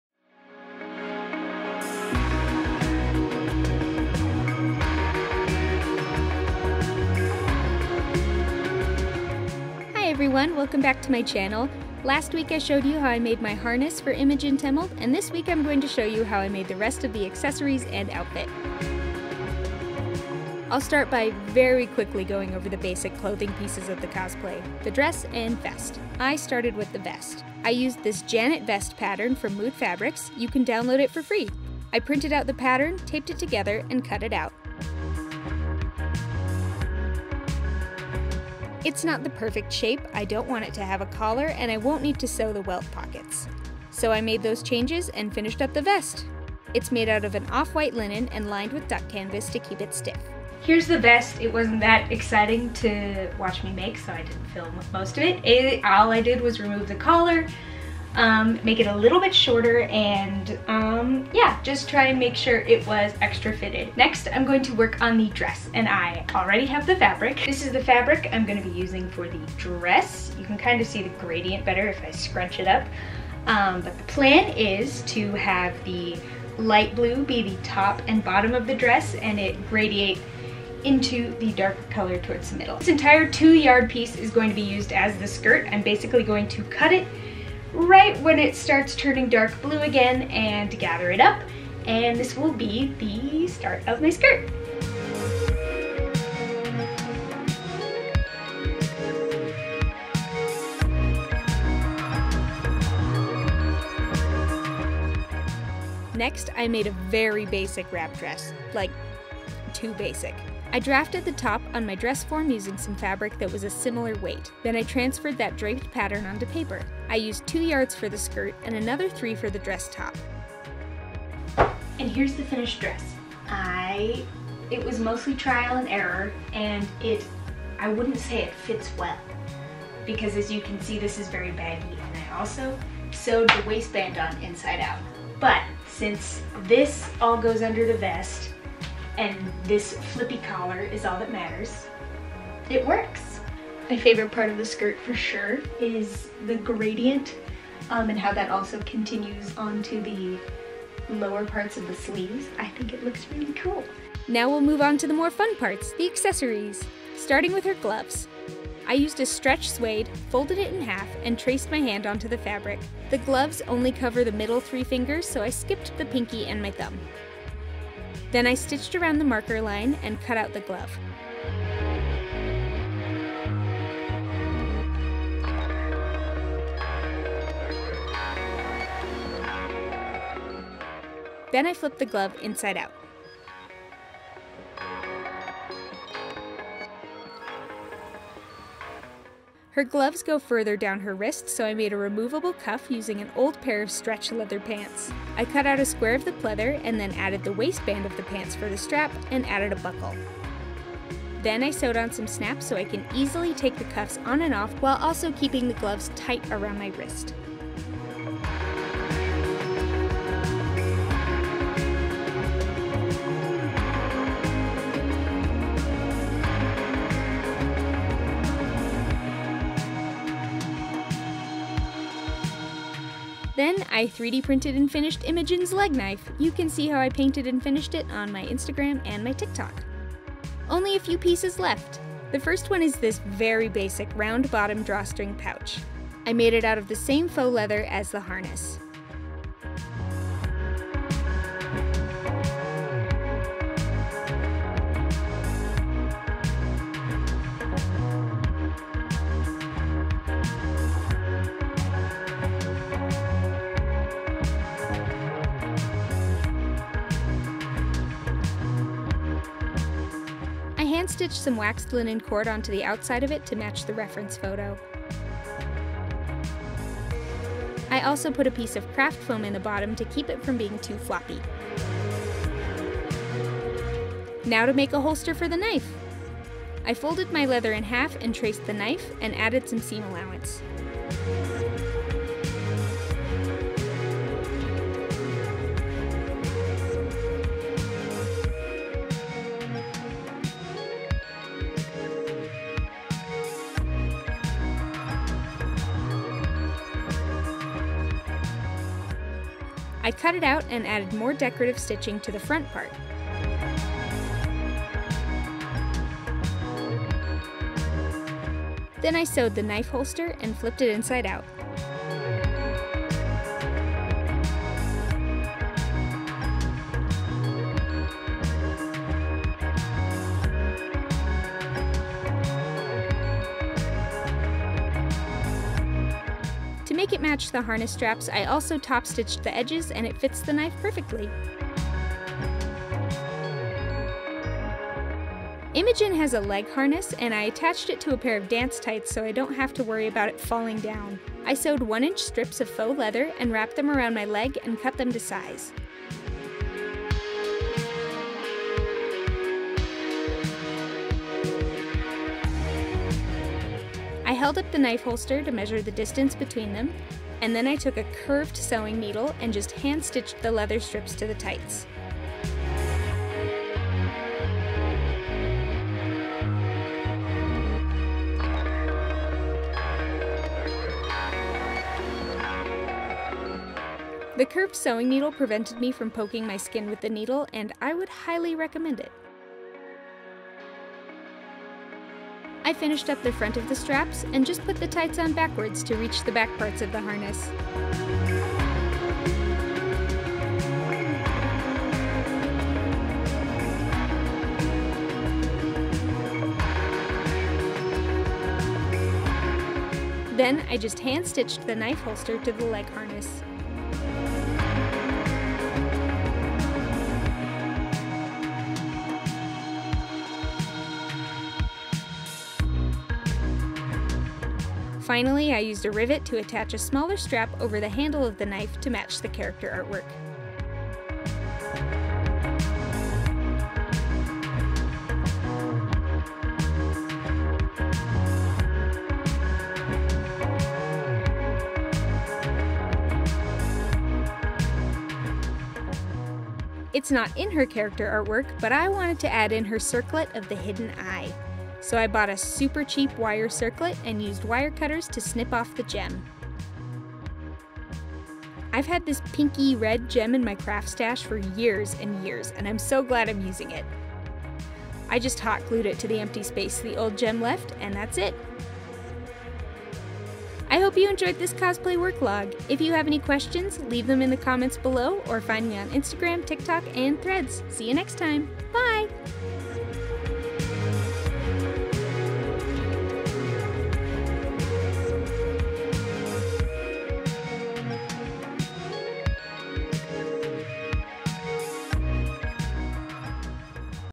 Imogen Temult Worklog | Critical Role Cosplay
This is part 2 of my Imogen Temult cosplay worklogs. In this video I cover the vest, dress, and all the accessories!

Laudna is the lovely @Vanillaguard

Find me!